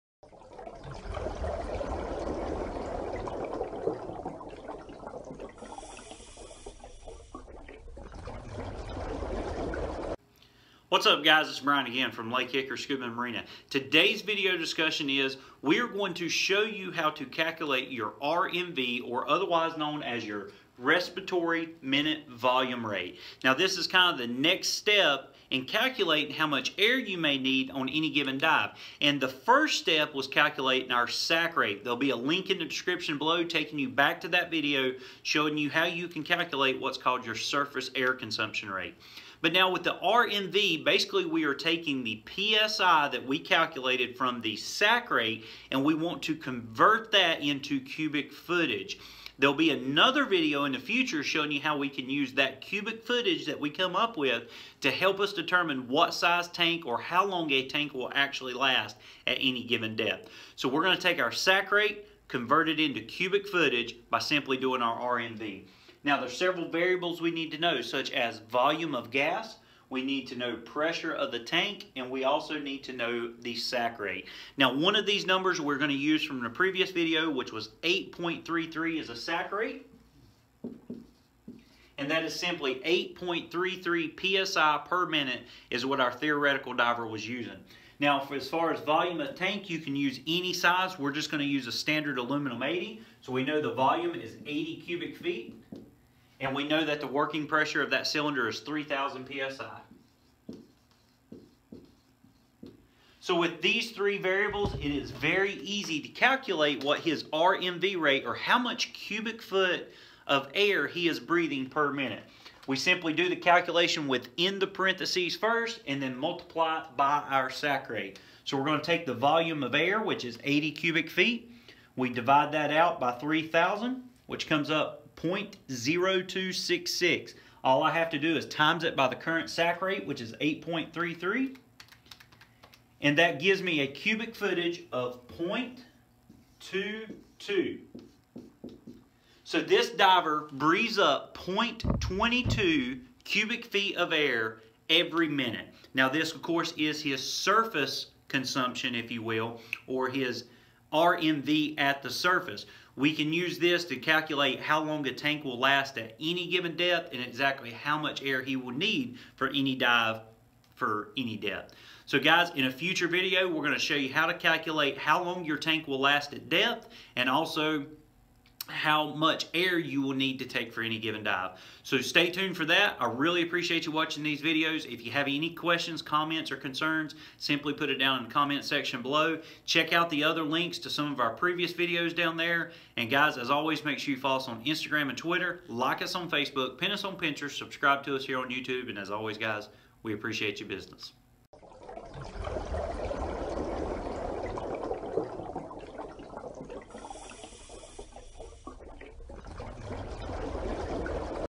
How Many Cubic Feet Of Gas Do You Breath? RMV RATE Explained
Once divers calculate how much PSI they breathe per minute, the next step is converting that into Cubic Feet.  Depending on what size cylinder they use, the formula is very easy to do.  As long as you have several variables.  Cylinder Size, PSI Of Cylinder, and SAC Rate.

(Volume / PSI) X SAC = RMV